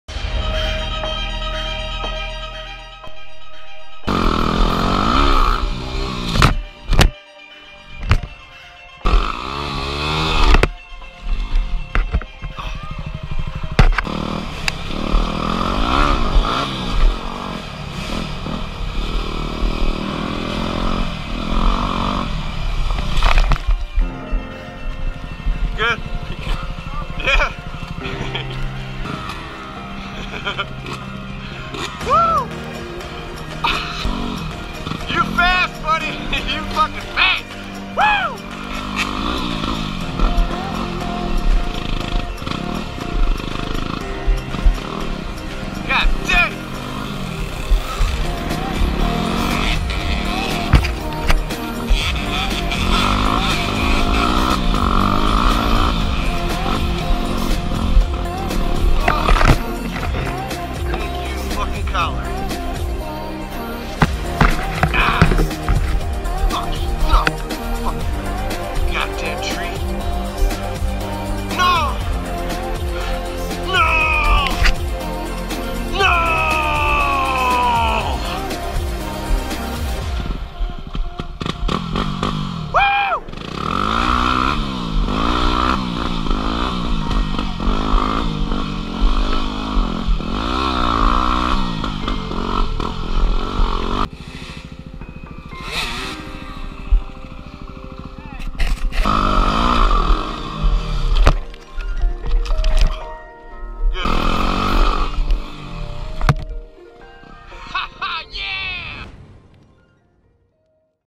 Mud moto at the Thunderdome!
Bonesaw and Marv125 rip it in the mud and rain